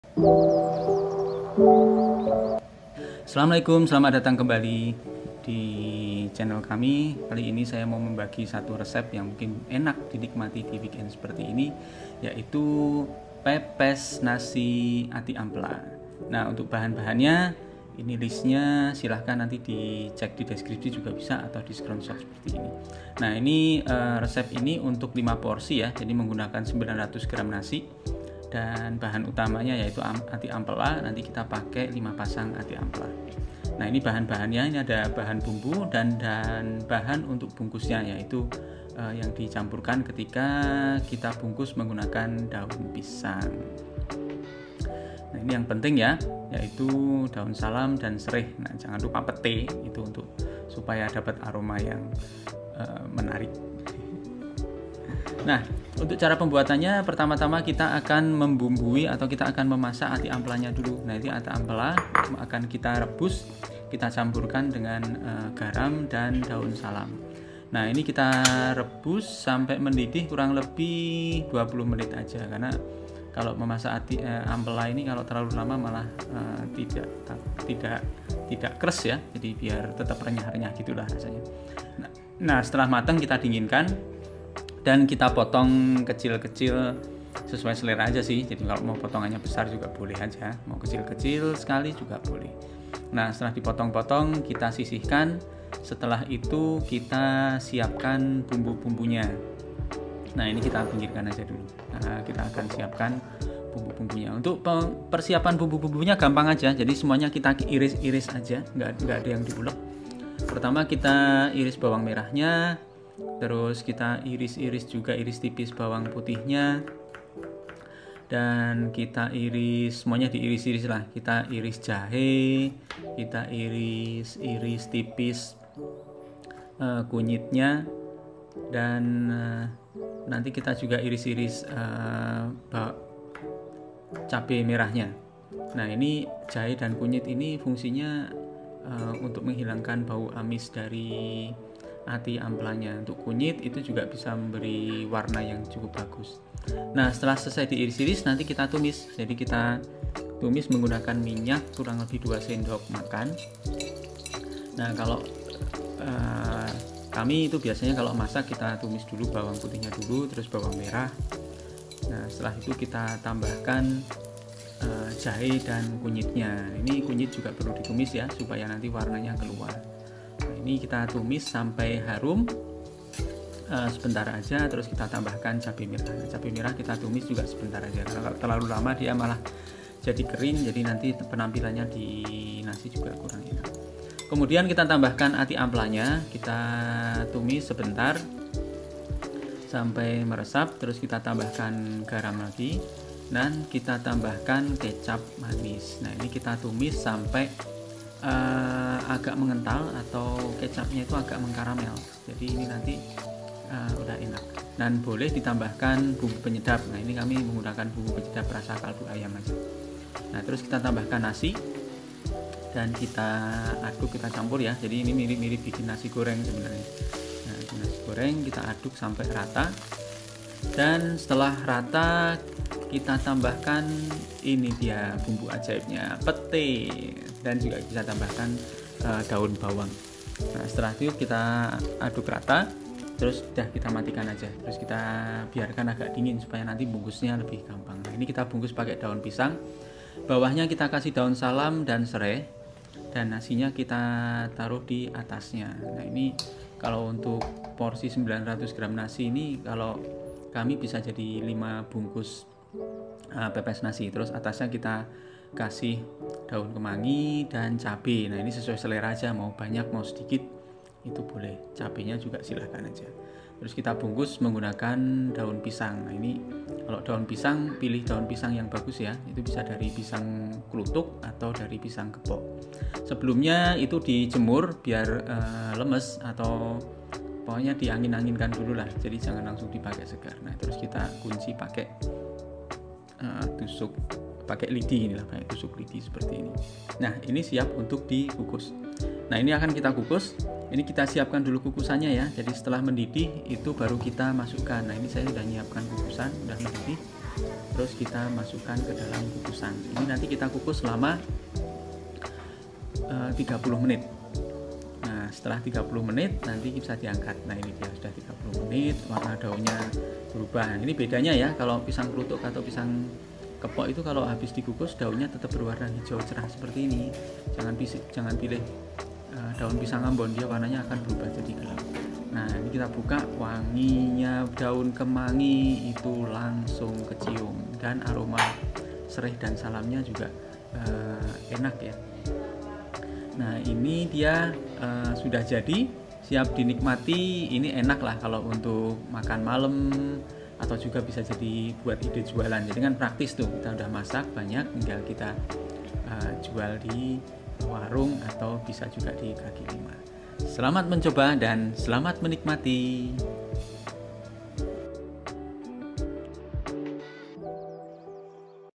Resep Nasi Pepes Ati Ampela | alternatif makan nasi enak
Bahan:

Bahan bumbu untuk 5 porsi:
- 900 gr nasi
- 5 pasang ati-ampela
- 60 gr (10 siung) bawang merah
- 30 gr (6 siung) bawang putih
- 3 buah cabe merah
- 2 batang bawang daun
- 1 papan petai
- kunyit dan jahe sebesar jari
- 1 1/2 sdt garam
- 1 sdt penyedap
- 5 sdm kecap manis

Bahan bungkus:
- Daun pisang
- Sereh
- Daun salam
- Cab raiwit
- Daun kemangi